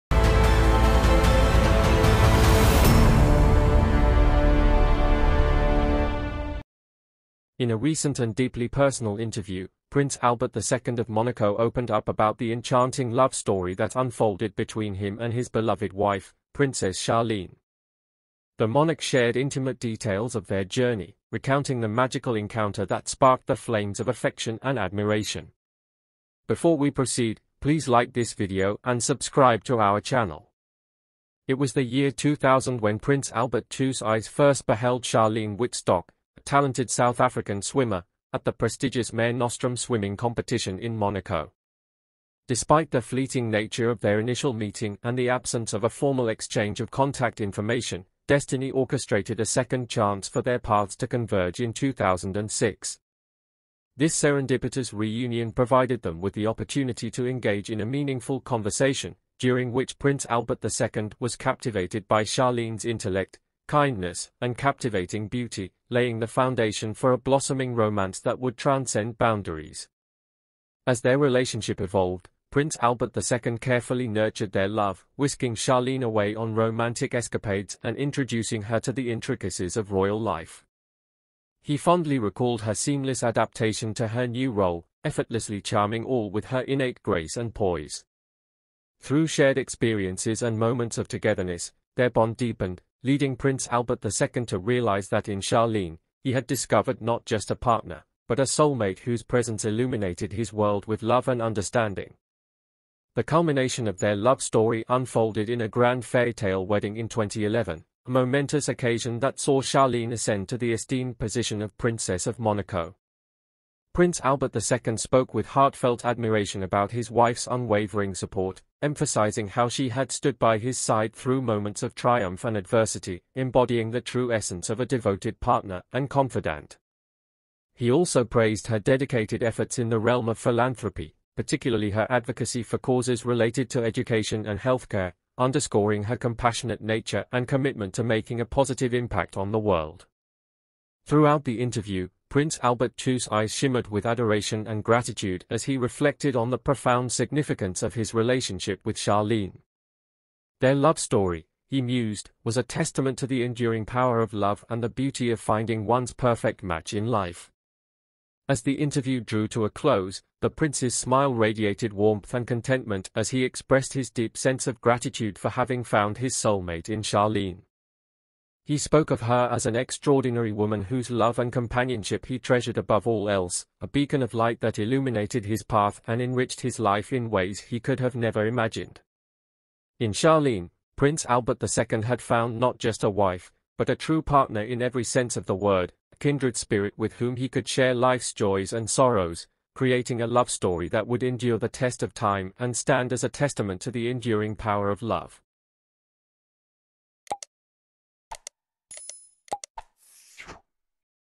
Prince Albert II Interview: How He Met Princess Charlene
Prince Albert II opens up about his first encounter with Princess Charlene in a heartwarming interview. Watch to find out what he had to say!